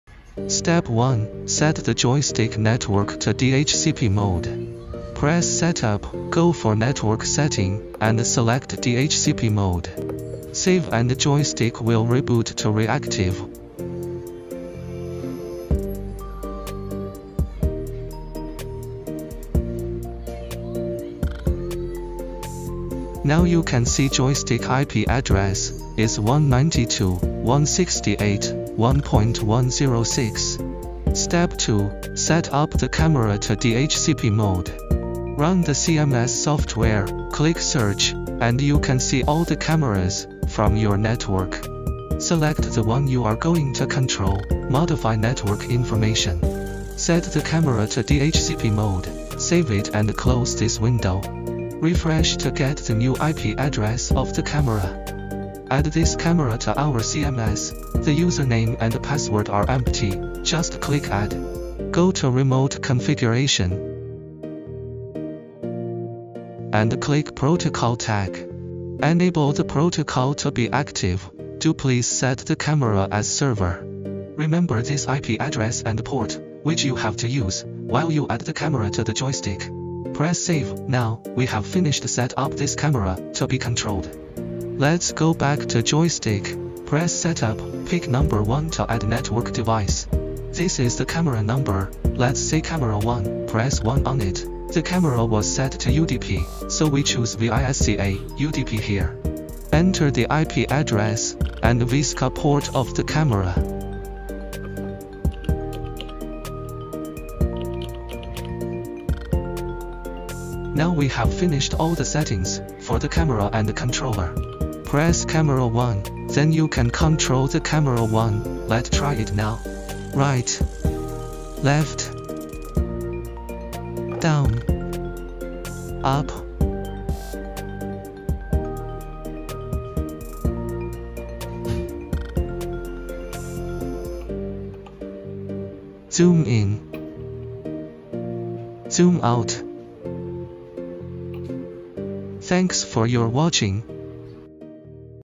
AVKANS Auto tracking PTZ camera settings for joystick
This is how to set up auto tracking ptz camera to be controlled by joystick over IP.